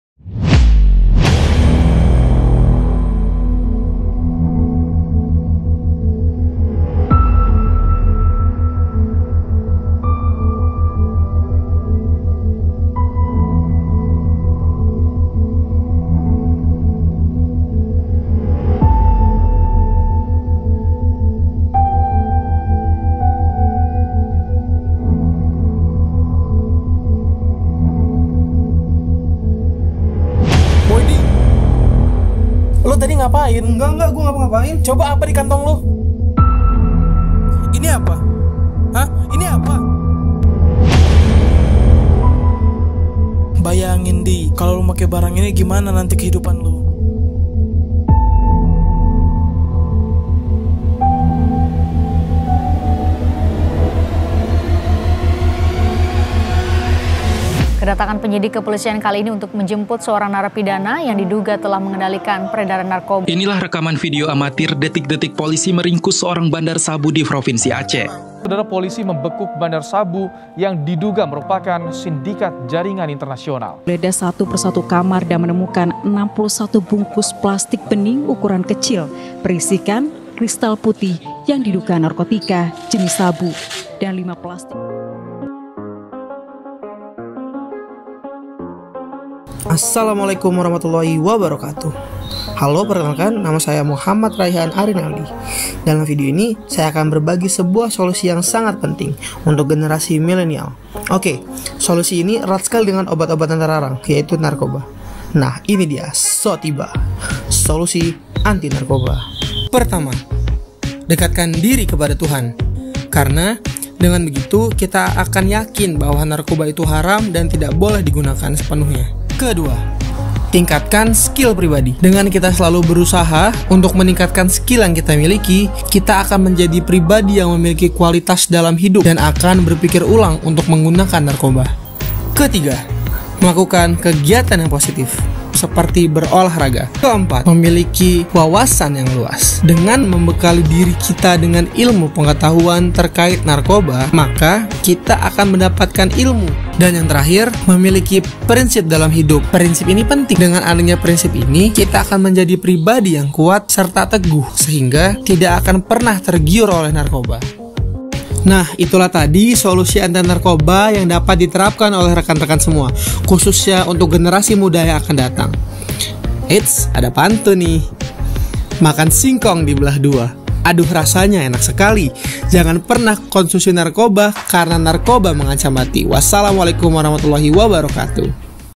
20 Besar Kompetisi Vlog CETAR 2021 | @aldii.han | Cegah Tangkal Narkoba | Kategori Mahasiswa
CETAR adalah bentuk kampanye pencegahan penyalahgunaan narkoba dalam upaya mewujudkan program Keluarga Indonesia Sehat Tanpa Narkoba (KRISAN) yang diselenggarakan oleh Tim Penggerak PKK Pusat Pokja 1.

Video blog ini merupakan peserta 20 besar Kompetisi Vlog CETAR 2021 Kategori Mahasiswa yang dibuat oleh Muhammad Raihan Arinaldi asal Lampung.